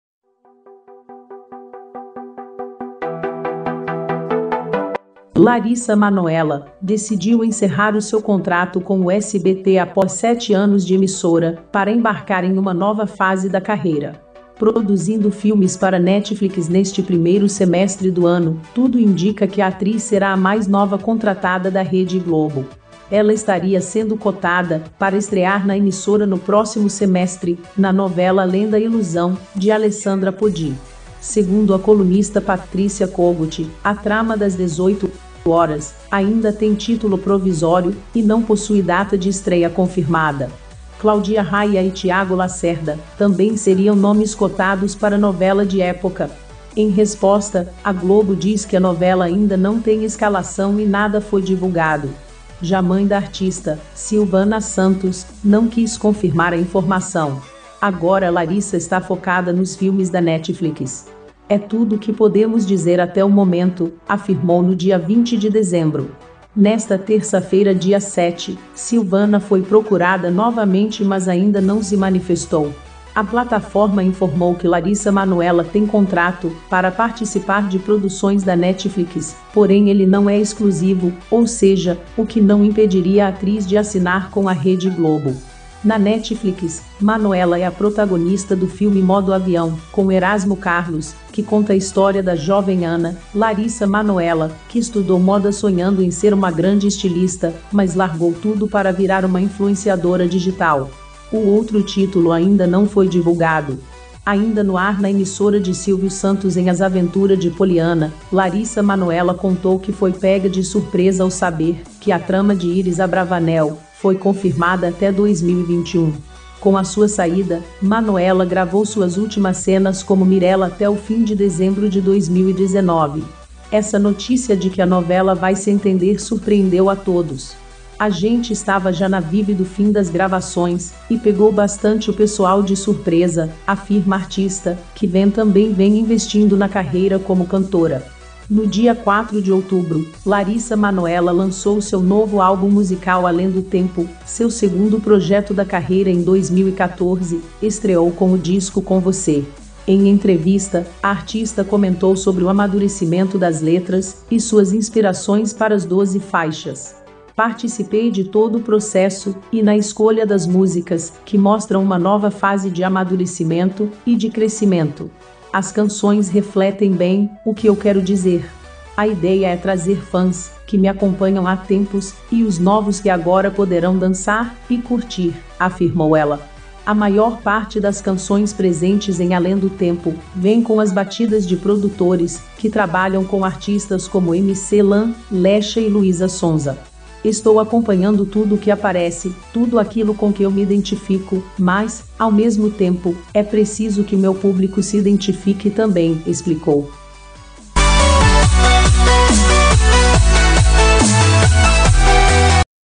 Larissa Manoela estará em novela da Globo.
Larissa Manoela, 18, decidiu encerrar o seu contrato com o SBT após sete anos de emissora para embarcar em uma nova fase da carreira.